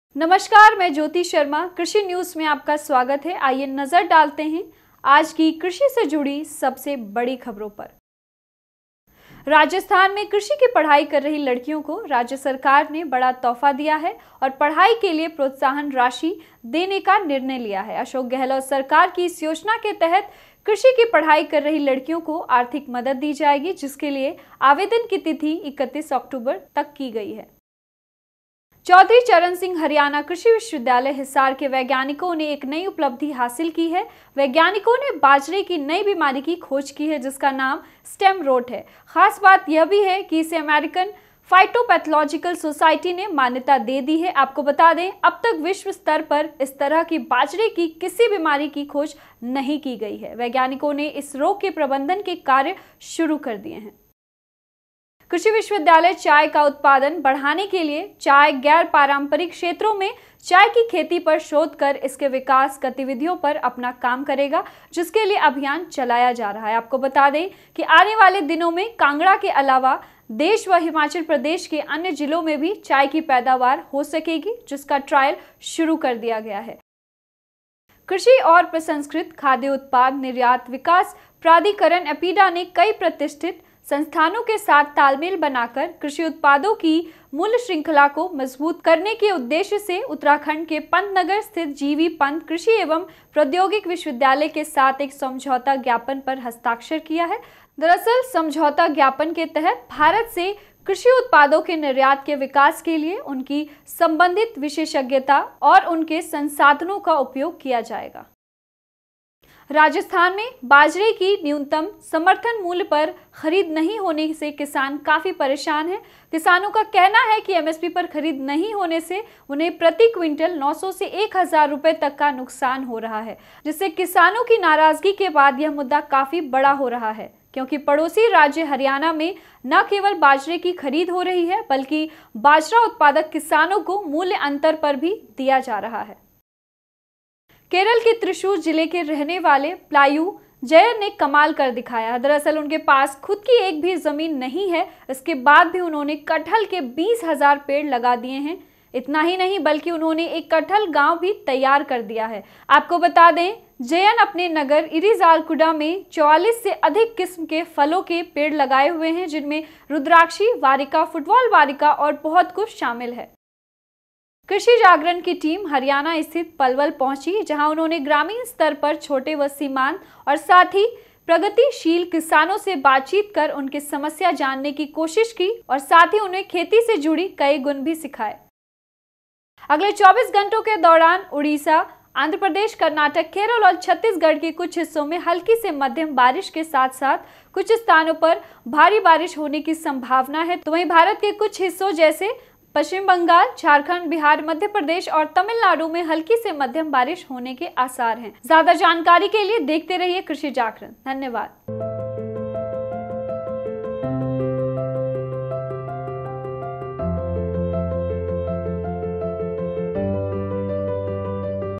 पढ़ाई के लिए प्रोत्साहन राशि | कांगड़ा में चाय उत्पादन | करेल में कटहल का गांव | HAU Hisar | APEDA
आज के कृषि न्यूज में देखिए राजस्थान की लड़कियों को मिला बड़ा तोहफा, एचएयू हिसार के वैज्ञानिकों बाजरे की नई बीमारी की खोज, देश के गर्म इलाकों में भी होगा कांगड़ा चाय का उत्पादन, एपीडा ने उत्तराखंड के दो संस्थानों से किया करार, कटहल के 20, 000 पेड़ लगाकर बना दिया कटहल गांव, राजस्थान में बाजरे की MSP पर खरीद ने होने किसान परेशान।

FTB Organic